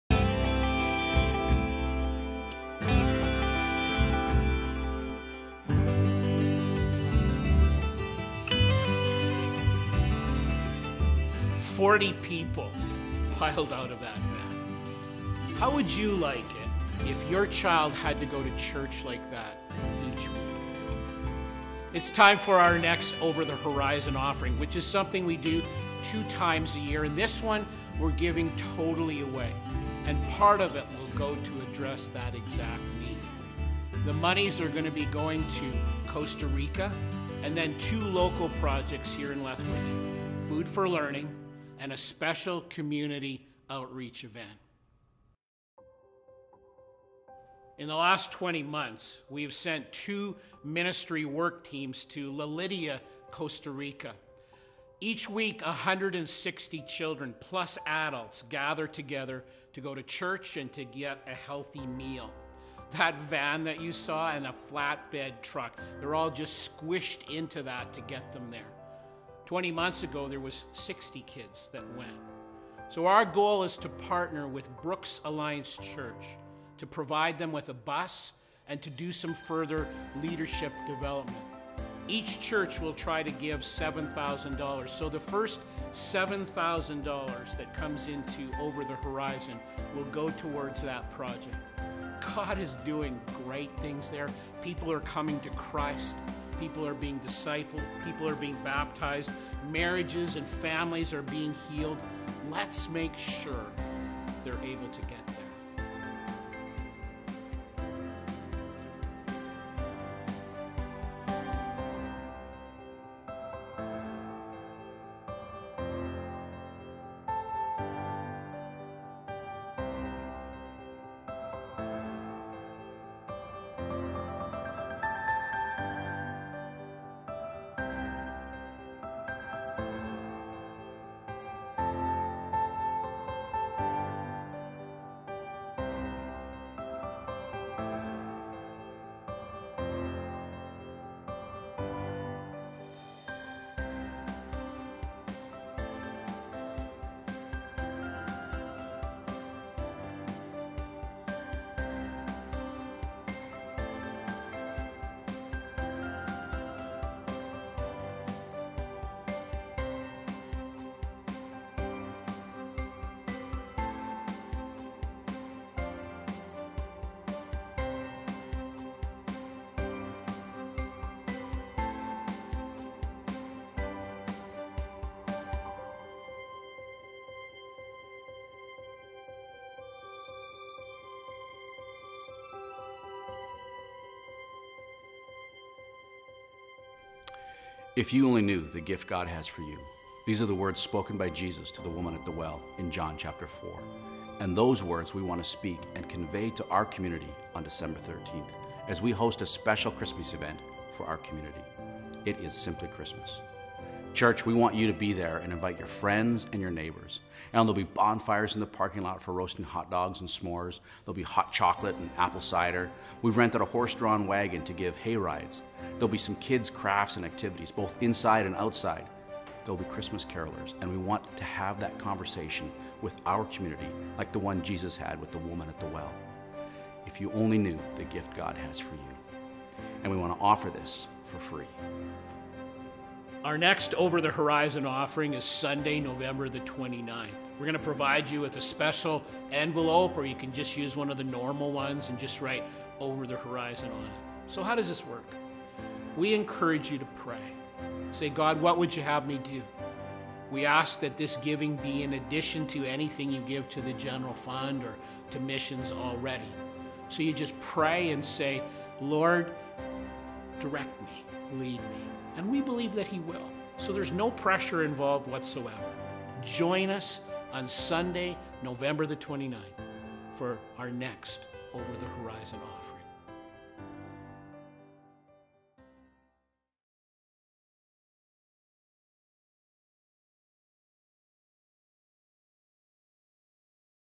Over the Horizon - November 29, 2015
On November 29, 2015 partner with University Drive Alliance Church for our next Over the Horizon offering. The first $7000 will go to help La Lydia, Costa Rica and the remainder will be used to help Food 4 Learing and Simply Christmas, a community event.